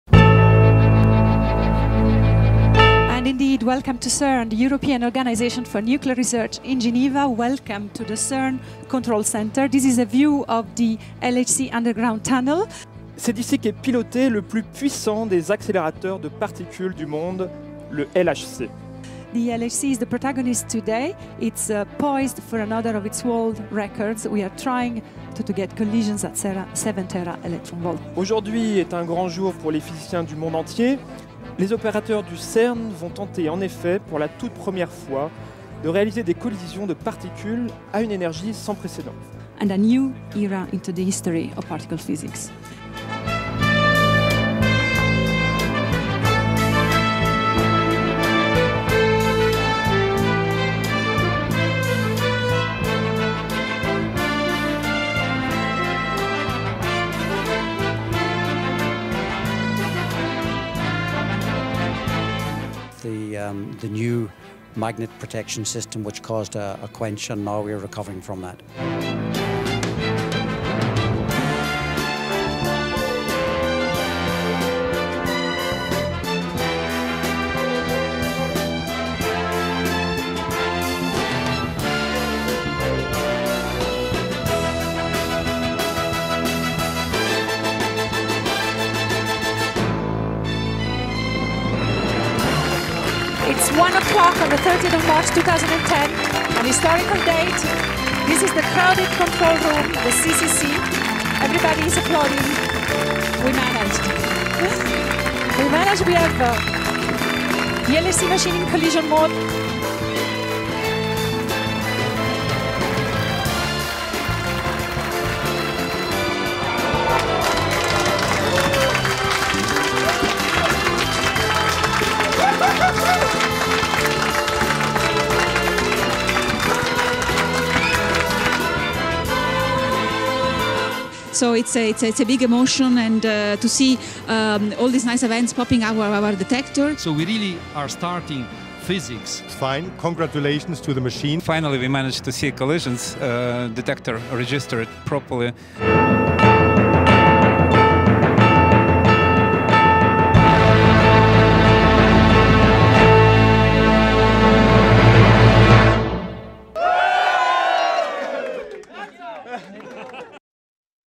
10 years ago, the first high-energy collisions at the LHC.
🍾 OnThisDay, exactly 10 years ago, the Large Hadron Collider – CERN’s flagship accelerator – embarked on its high-energy voyage.

🖥 Relive the moments leading up to the first high-energy collisions at the LHC.

And find out more in the first feature from our series celebrating